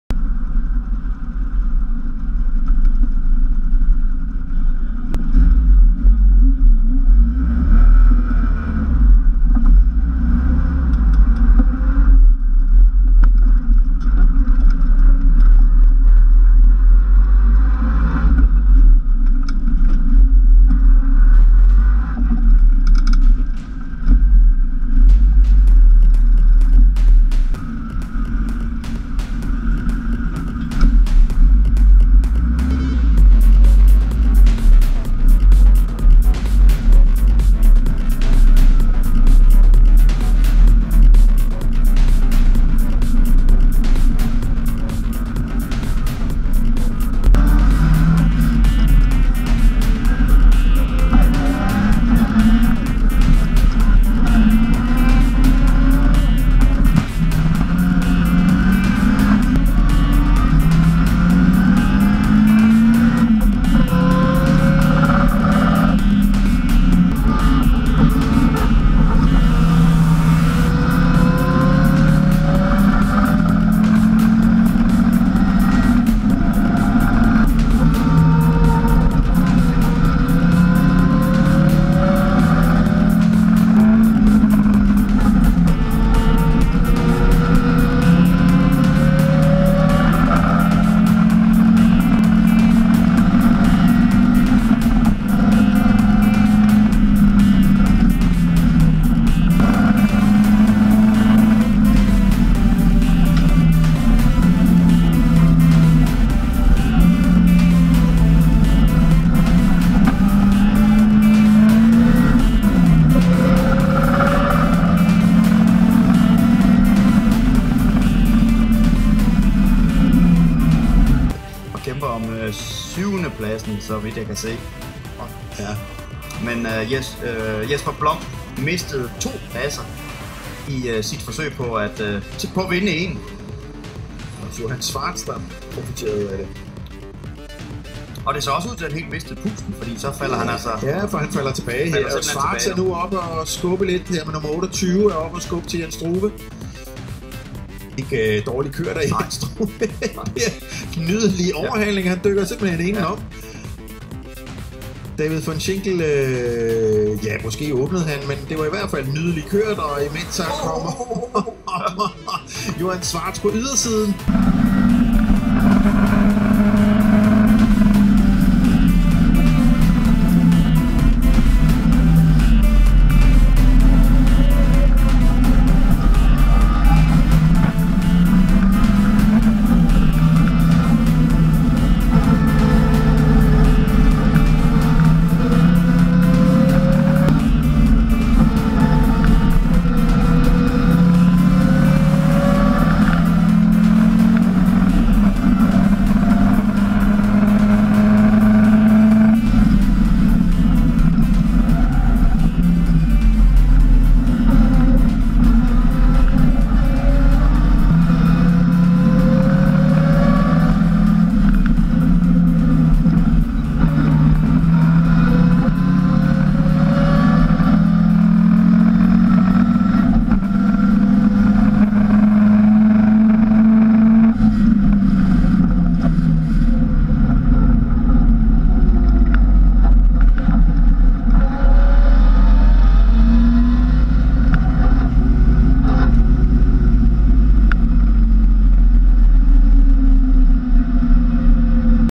Johan Schwartz Lotus Cortina Copenhagen Historic Grand Prix 2017
Copenhagen Historic Grand Prix Sunday Pro-Am Race August 6th, 2017